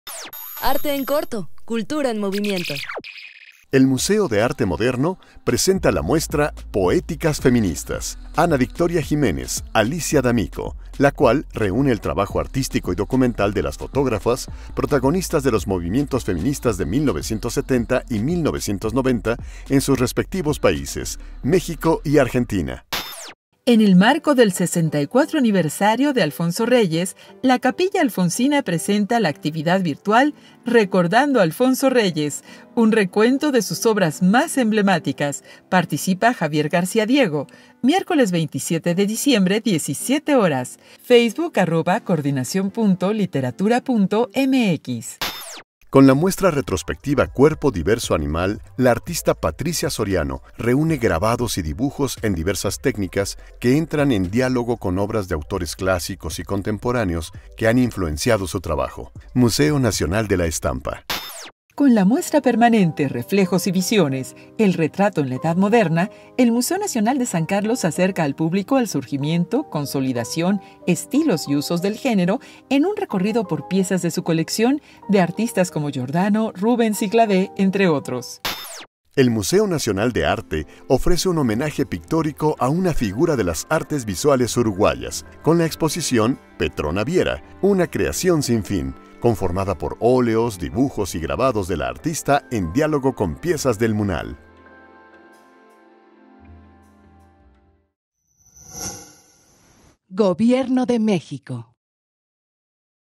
Arte en corto 243. Cultura en movimiento
En nuestro noticiero ArteEnCorto de esta semana conocerás la oferta cultural que el Museo de Arte Moderno de México, la Coordinación Nacional de Literatura, la Capilla Alfonsina, el Museo Nacional de la Estampa MX, el Museo Nacional de San Carlos y el Museo Nacional de Arte INBA tienen para ti. 🔴🎥😉